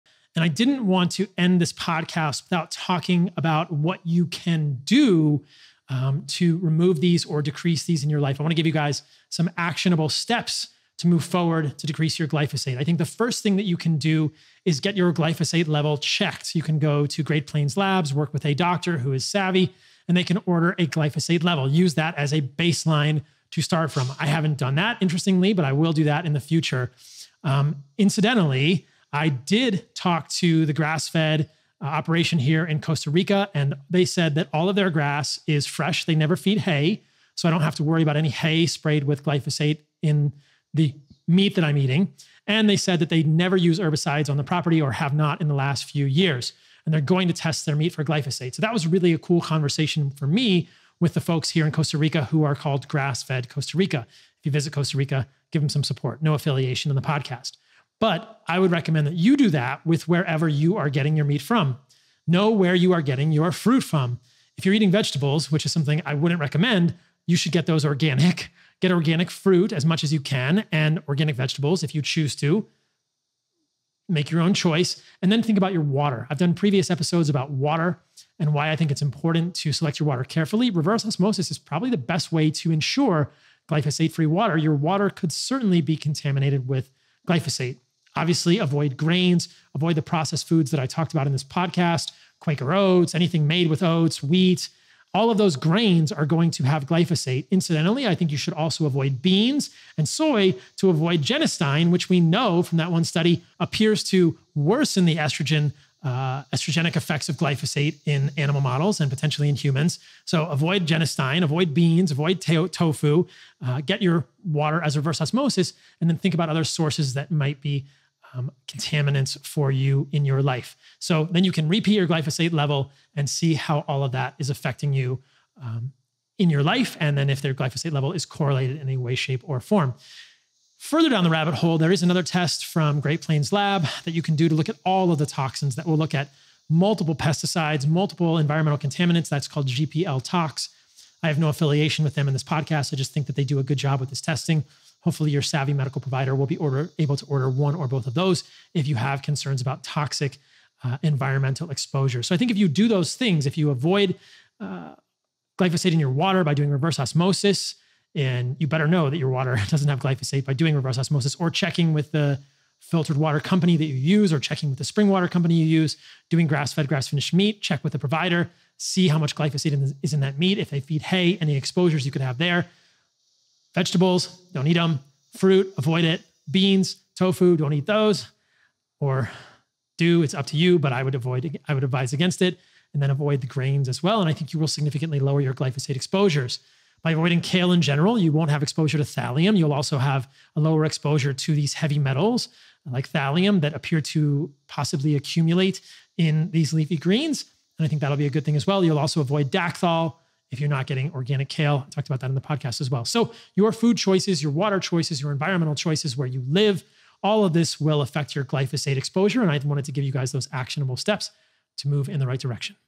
How to lower glyphosate exposure
Paul shares actual steps you can take to avoid glyphosate in your water and food in terms of things to avoid and things to consider in your food sources.